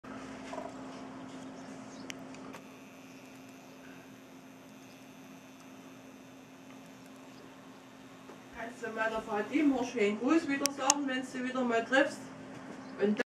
Auf, auf...ins Feld!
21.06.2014 ... Durch das Fenster gefilmt... Der Raps ist vom Hubschrauber flach gedrückt, der vor Wochen schon hier gelandet ist.